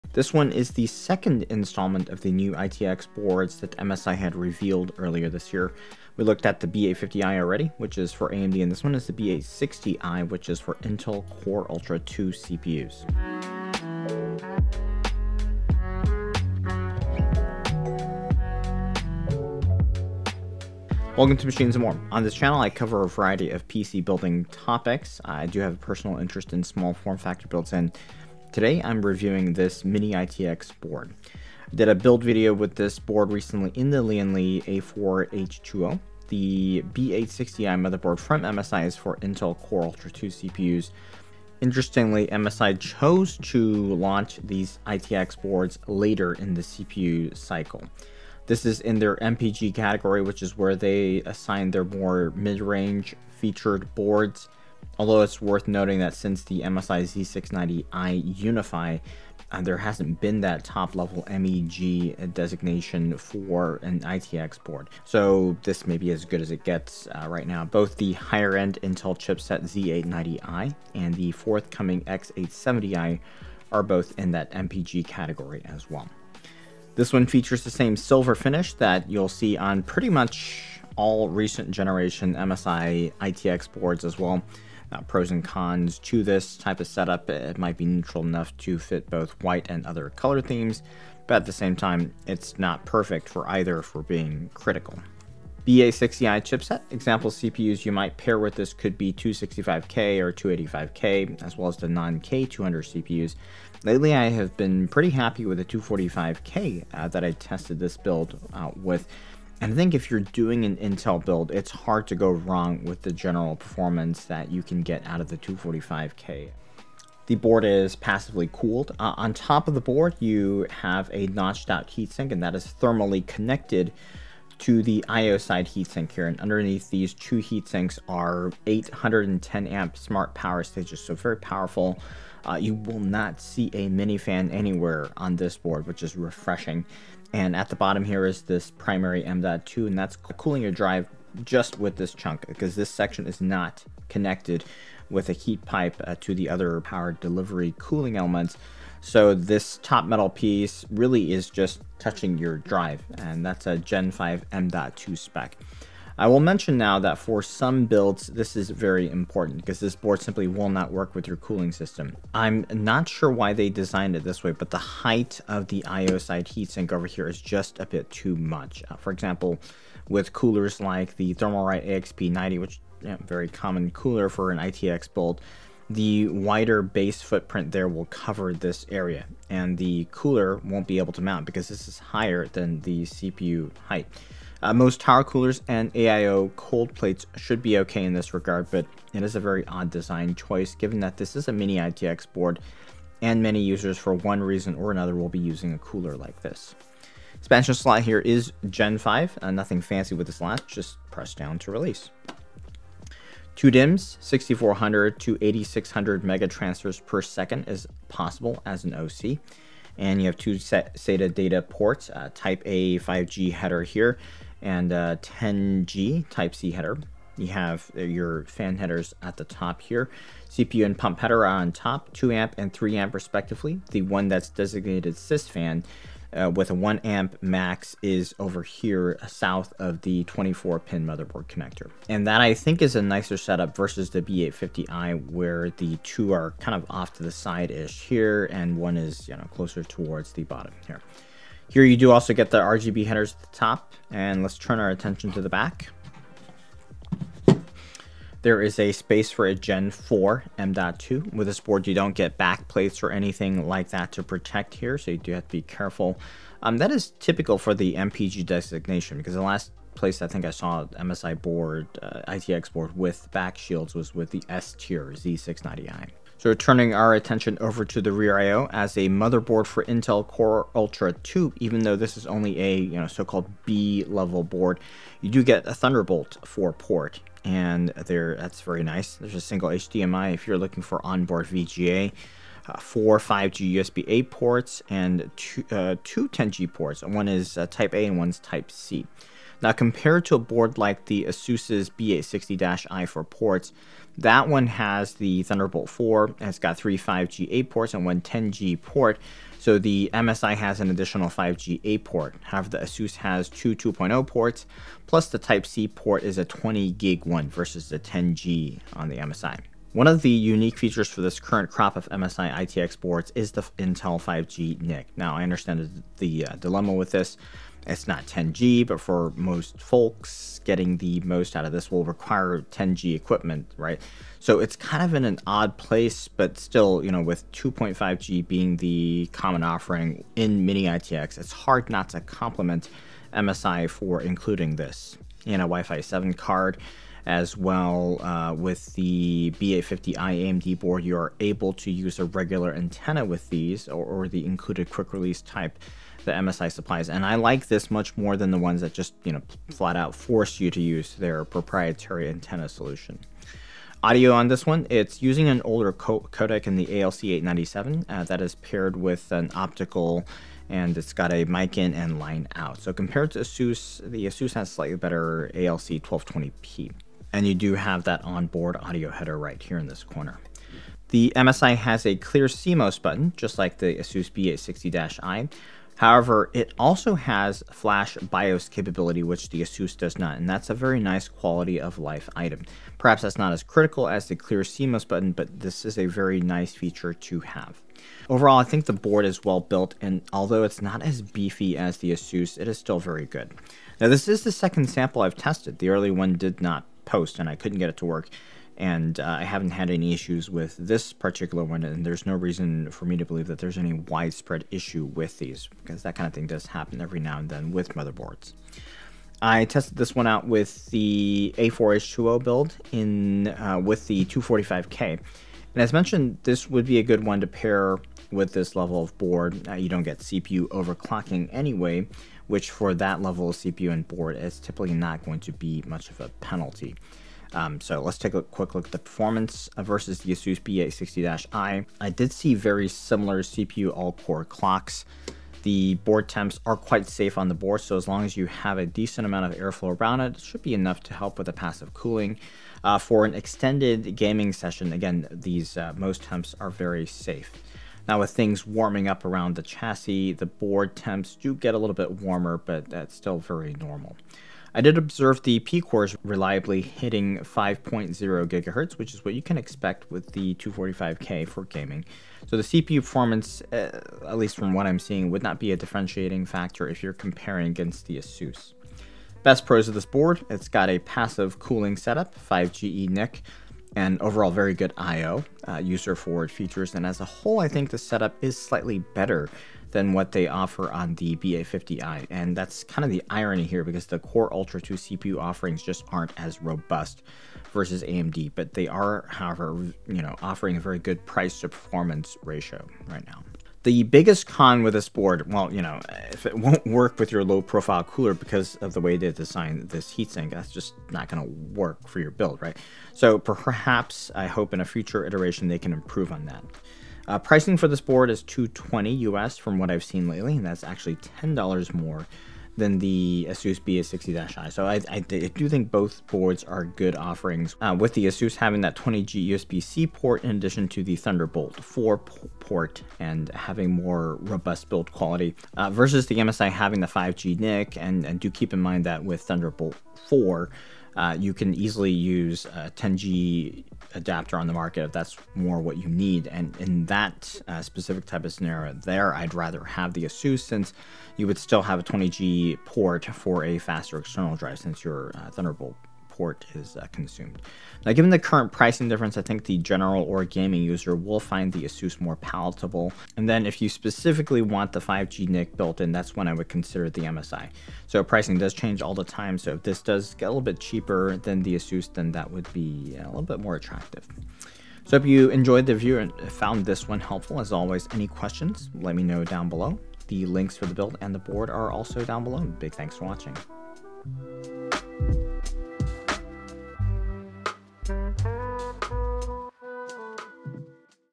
MSI B860I Edge - Big features on a little board! (some too big...)
MSI's B860I Edge is a mid-cycle mini ITX entry into their Intel Core Ultra 2 motherboard lineup. This one is a solid board with great features, including a 5G NIC - but it has one major catch to be aware of.
Lian Li A4-H2O Build
AIO Cooler
M.2 (2TB)

0:00 Intro
1:38 Board Features
2:40 A Tall Problem
4:30 Rear IO
6:52 Performance Testing Experience
7:52 Pros and Cons
8:31 Pricing and Comparison